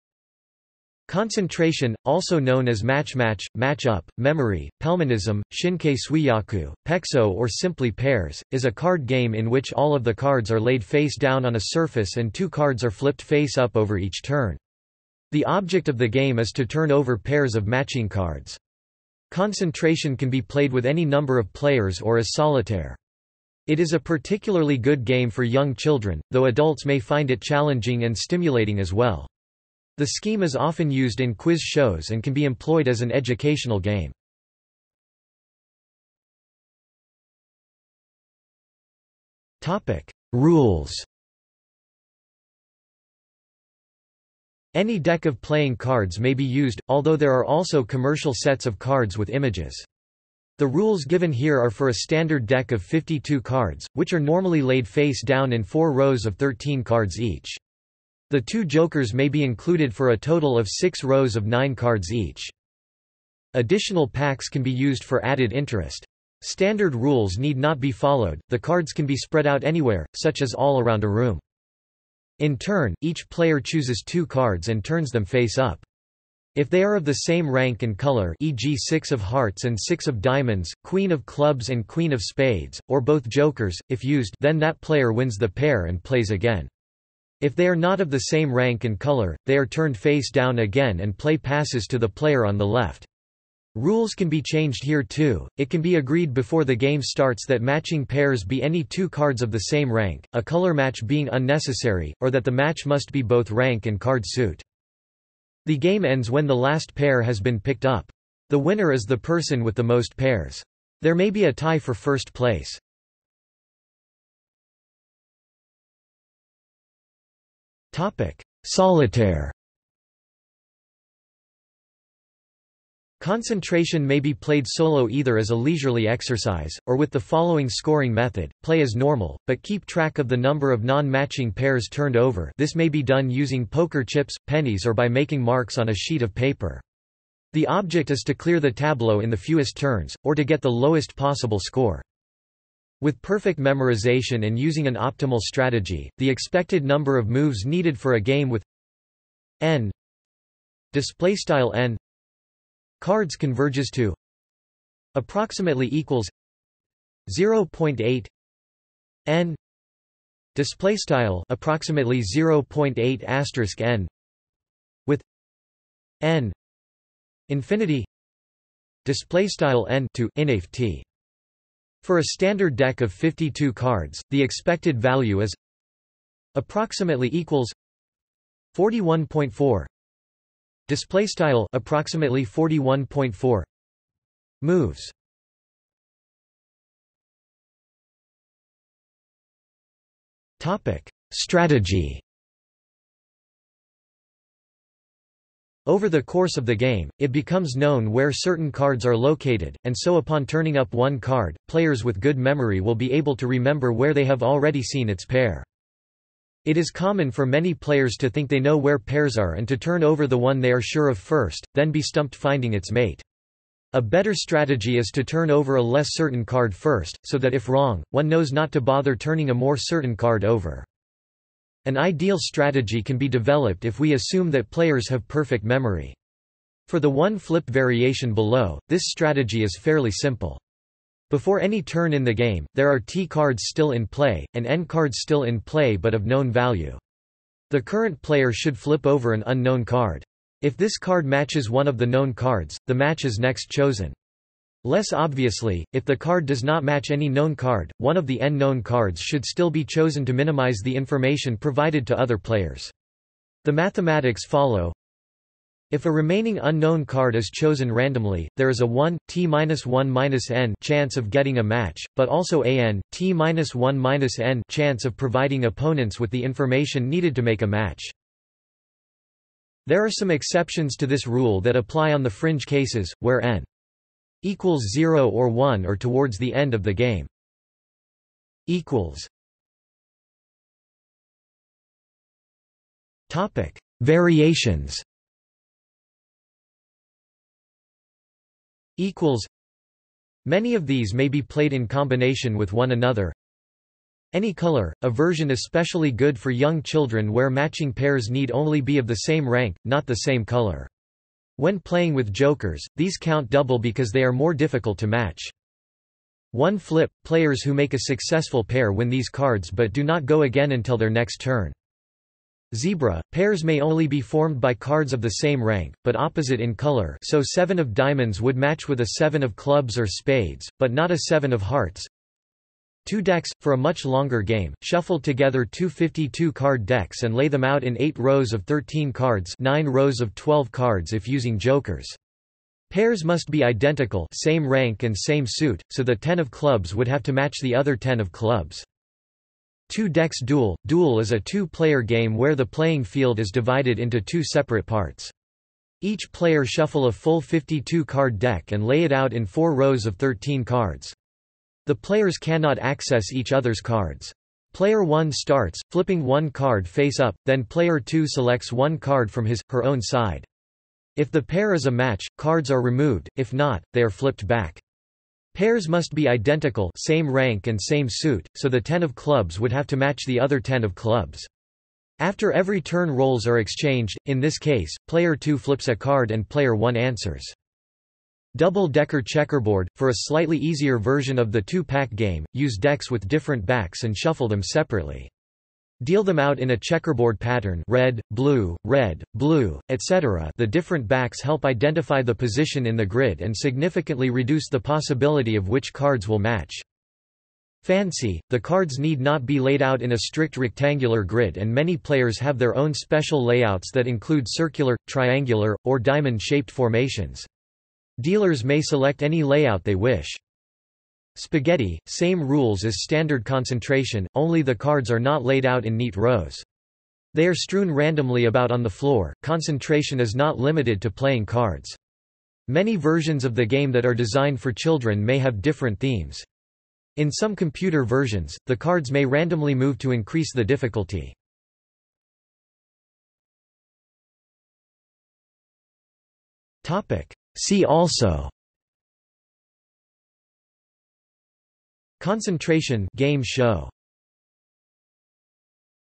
Concentration (game) | Wikipedia audio article
00:00:36 1 Rules
00:01:54 2 Solitaire
00:03:07 3 Strategy
00:04:34 4 Variations
00:04:39 5 See also
00:07:13 6 References

- Socrates

SUMMARY
Concentration, also known as Match Match, Match Up, Memory, Pelmanism, Shinkei-suijaku, Pexeso or simply Pairs, is a card game in which all of the cards are laid face down on a surface and two cards are flipped face up over each turn. The object of the game is to turn over pairs of matching cards. Concentration can be played with any number of players or as solitaire.  It is a particularly good game for young children, though adults may find it challenging and stimulating as well. The scheme is often used in quiz shows and can be employed as an educational game.